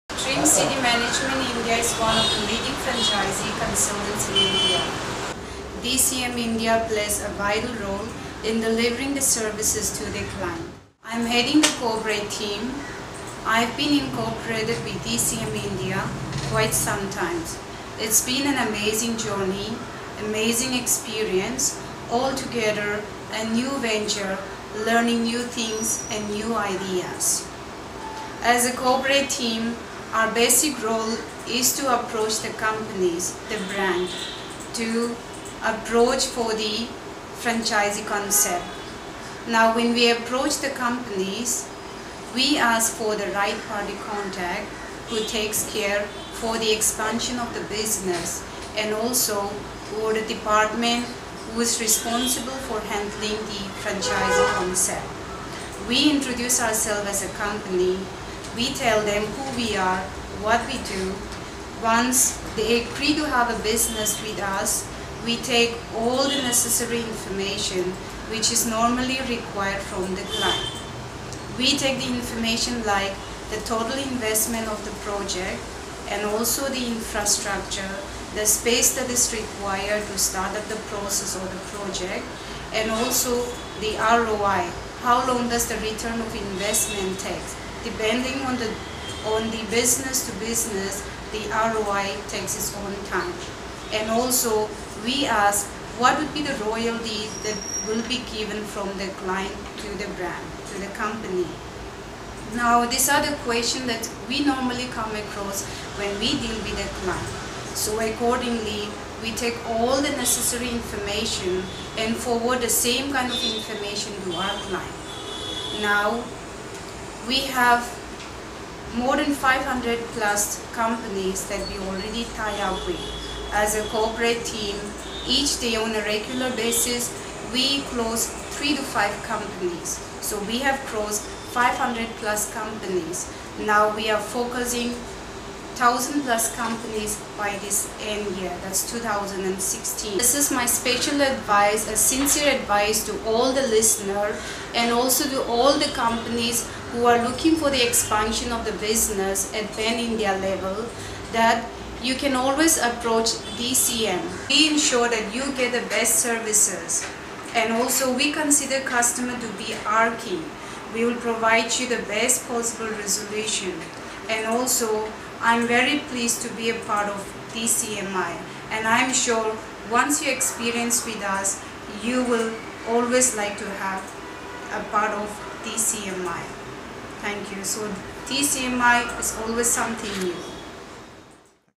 Benny review of DCM India | Dream city mangement India
benny is explaining how the DCM India works?
Dream city managemnt is one of the leading Franchise distributors in India. for more details log or call to toll free number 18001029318